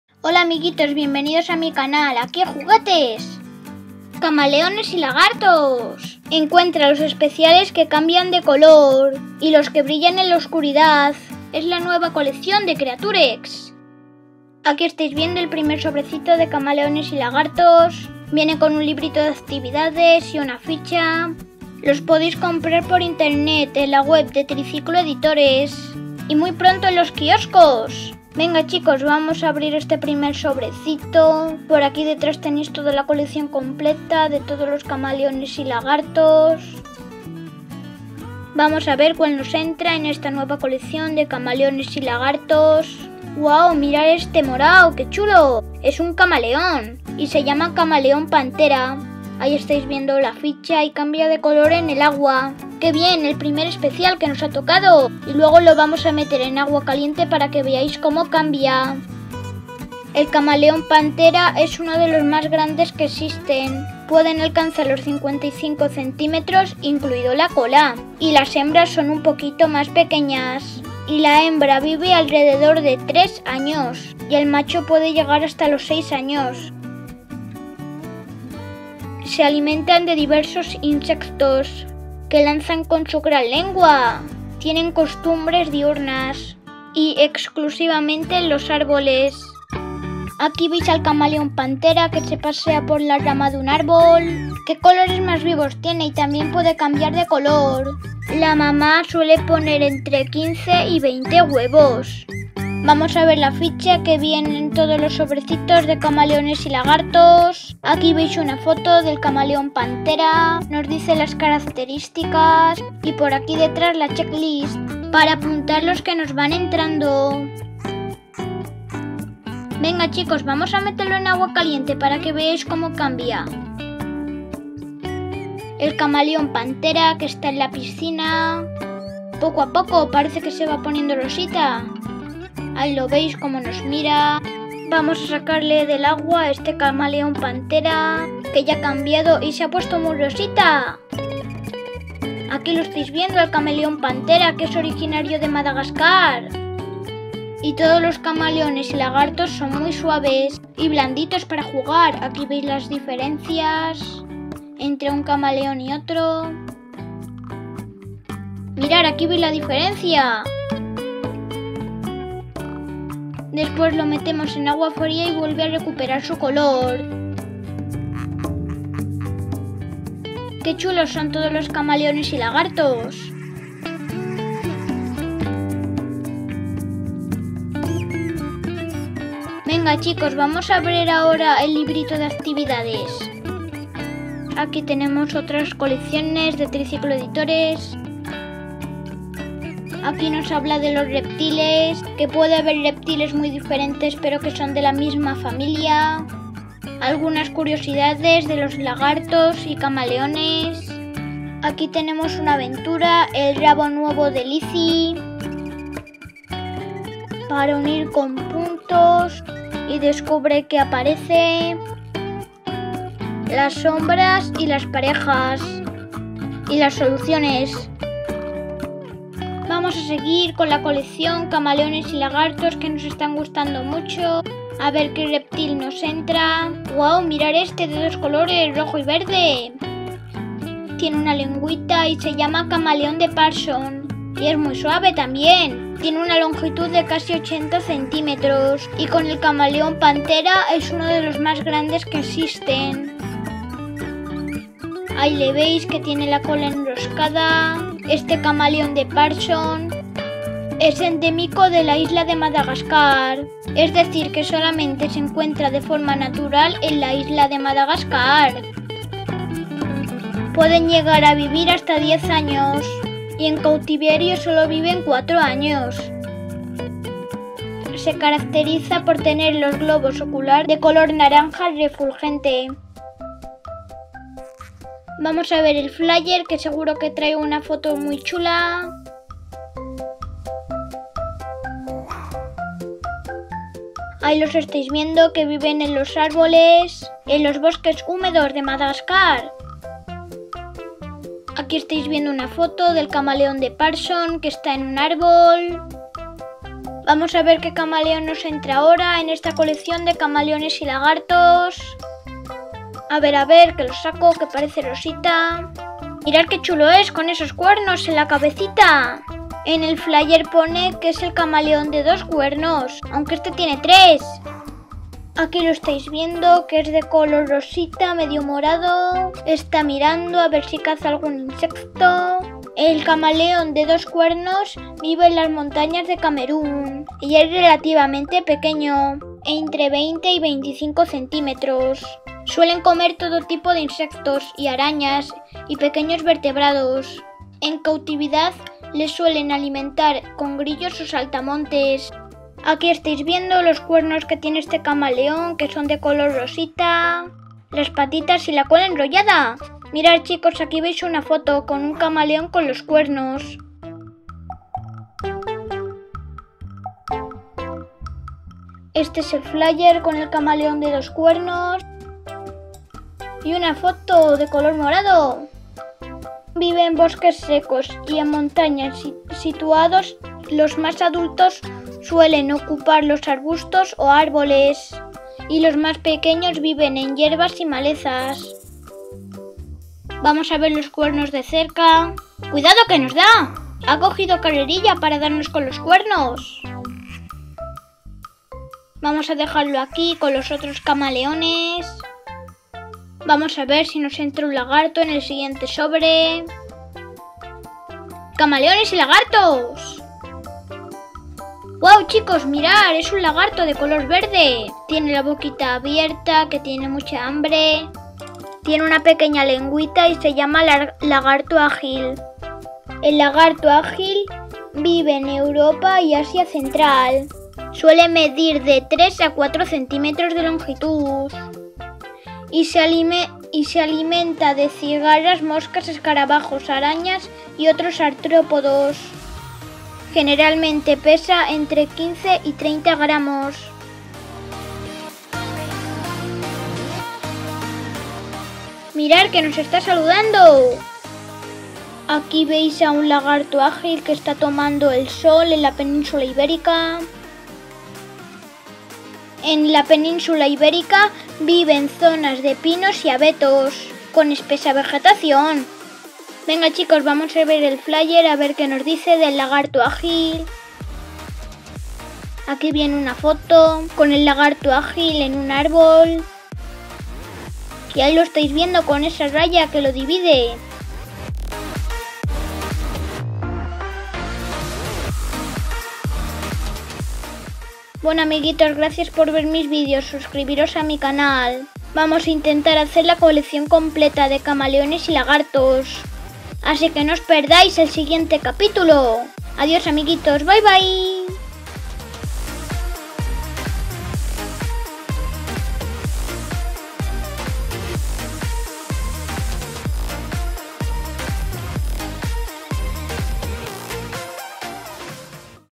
Kreaturex Camaleones y Lagartos sbabam. Encuentra Los Especiales.
Jelly Planet Camaleones y Lagartos encuentra los especiales que brillan en la oscuridad y los que cambian de color de sbabam Animales blanditos
Camaleones y Lagartos componen la colección:
    Camaleón de los tres cuernos
    Camaleón Pantera
    Camaleón Gigante de Madagascar
    Lagarto Ágil
    Lagarto Africano Arcoíris
    Lagarto de Gorguera
    Lagarto Analis Verde
    Lagarto de Cola Espinosa
    Camaleón Indio
    Camaleón de Meller
    Camaleón de Parson
    Camaleón de Jackson

AQUI JUGUETES CANAL DE JUGUETES Y DIVERSIÓN